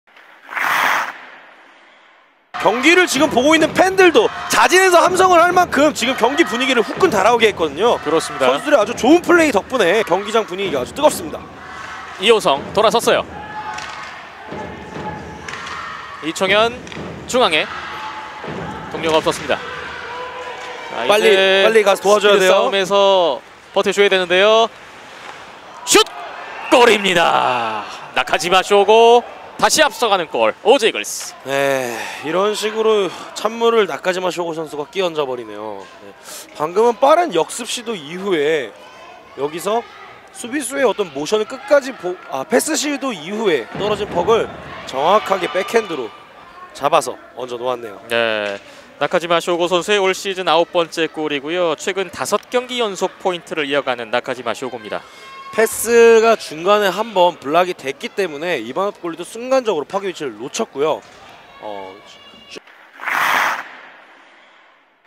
역습 상황에서 스스로 추가골을 만들어내는 오지이글스의 나카지마 쇼고 | 2020. 1. 19
아시아리그 아이스하키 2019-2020
대명킬러웨일즈 vs 오지이글스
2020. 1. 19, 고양